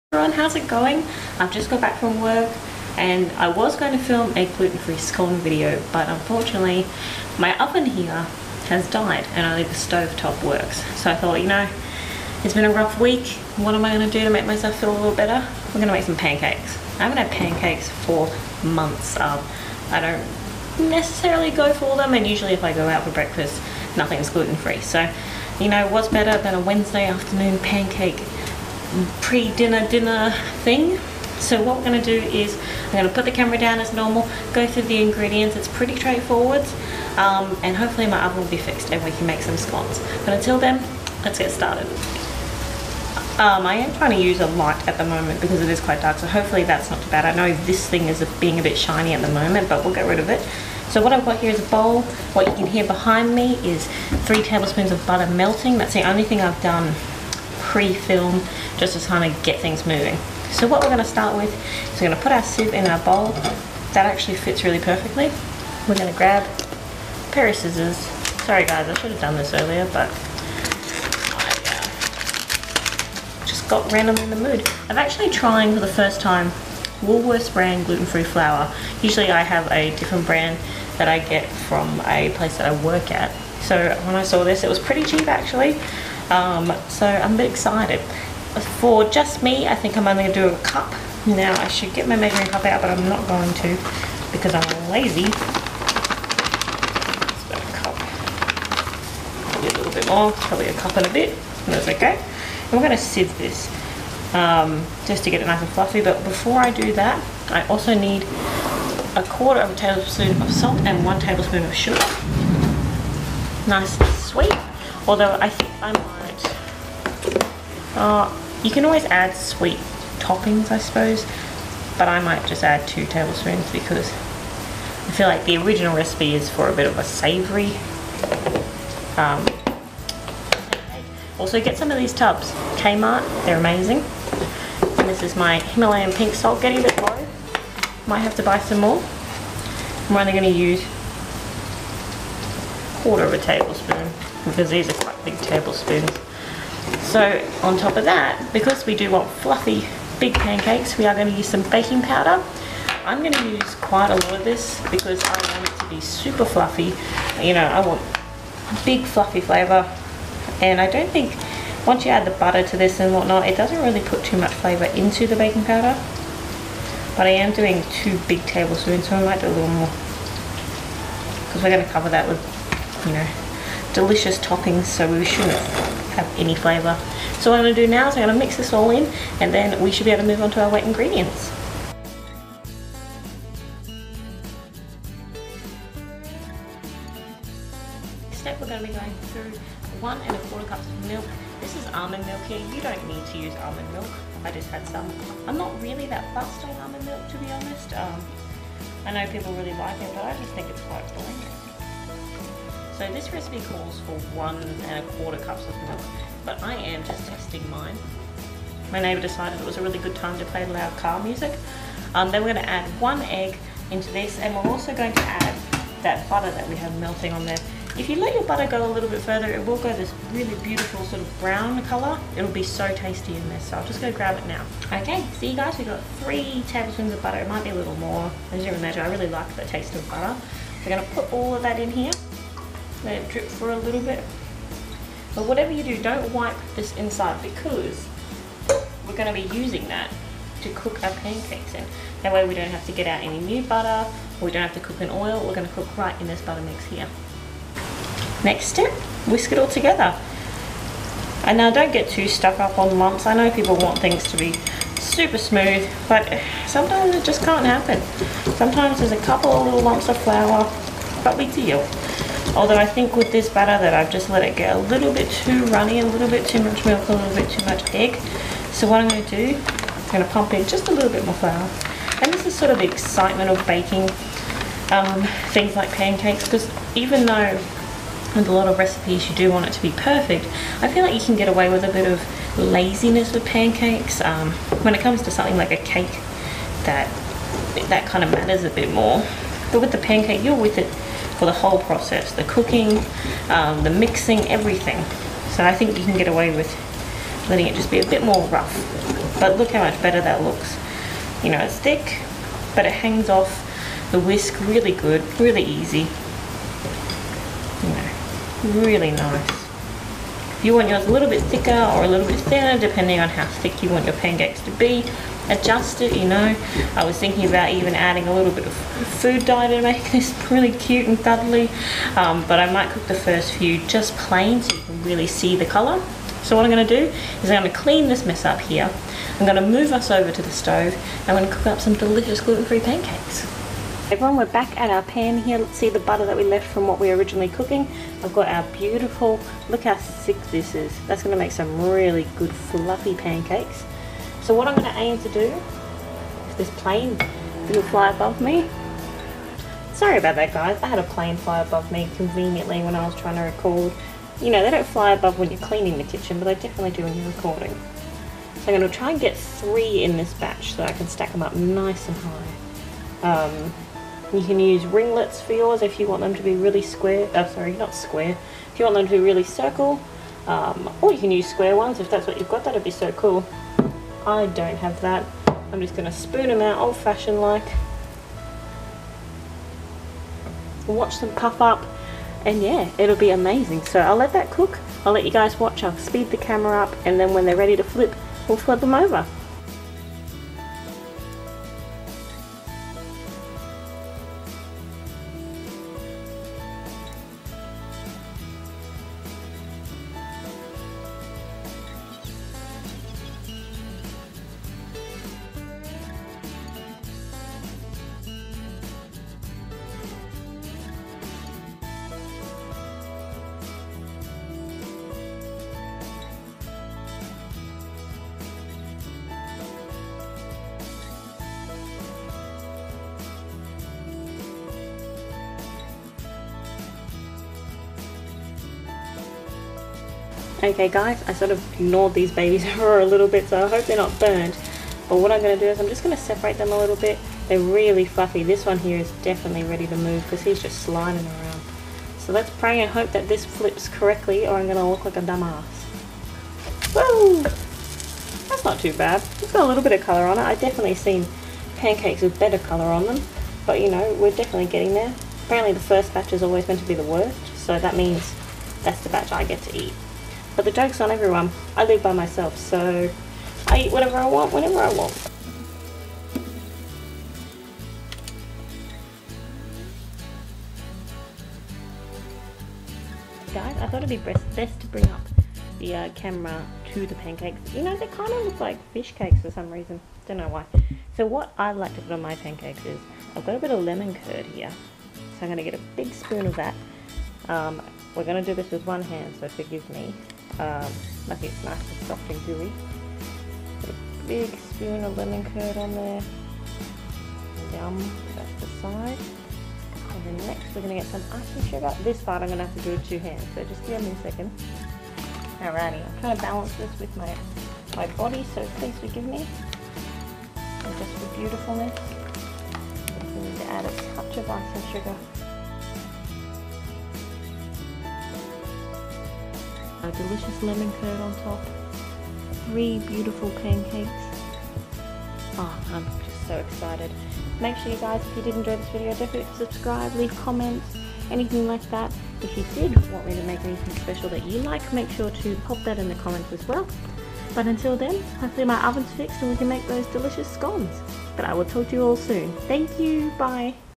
How to make gluten free fluffy pancakes.
Hope you enjoy these delicious, fluffy, gluten free pancakes. I think I had about seven of them!!

Enjoy everyone. :3